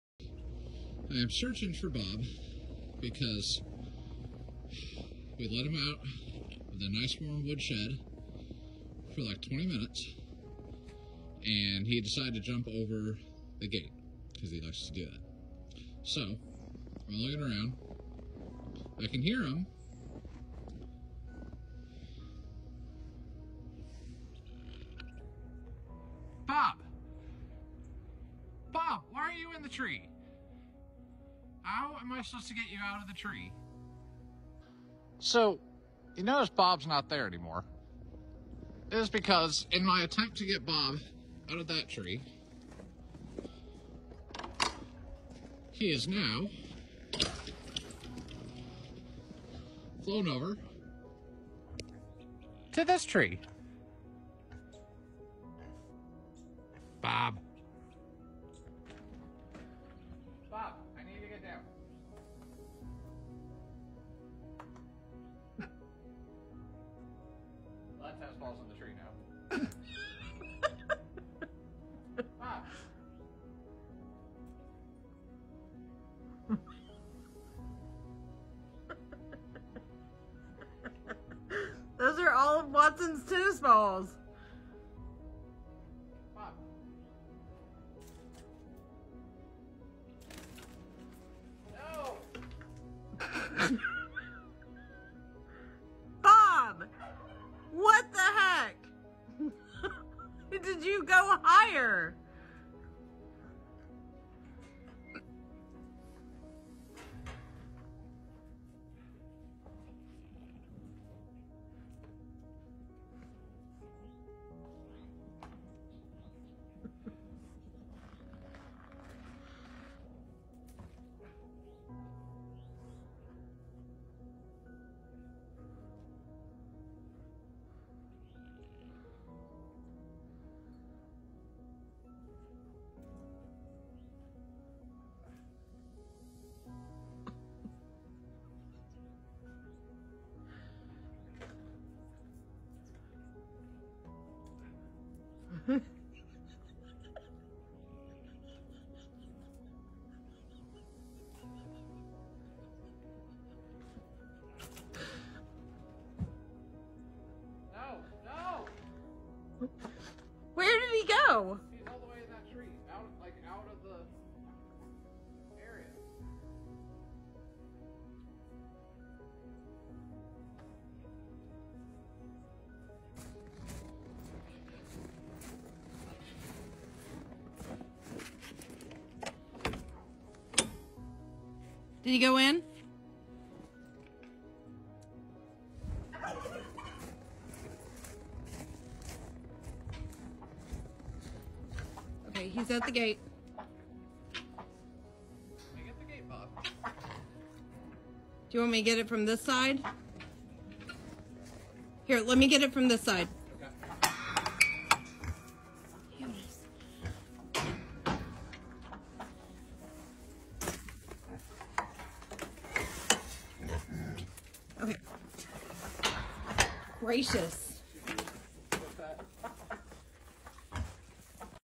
Bob the Guineafowl in the Snow | Fowl Friends Part 2
Meet Mr. Magerkel, the charming Blue Slate turkey, and his trusty sidekick, Bob the guineafowl! 🦃💙🐦 Their unlikely friendship not only warmed hearts but also saved Mr. M from a grim fate on the chopping block. 🙏 Now, Mr. M is a beloved sensation at the farm, captivating everyone with his majestic presence and winning personality. 🌟 And when it comes to the ladies, he's quite the show-off! 😉 Meanwhile, mischievous Bob keeps life interesting by constantly escaping the fence, leaving the farm staff on a wild goose chase (or should we say guinea chase!) 🏃‍♂️🏃‍♀️

Follow Fabled Farm!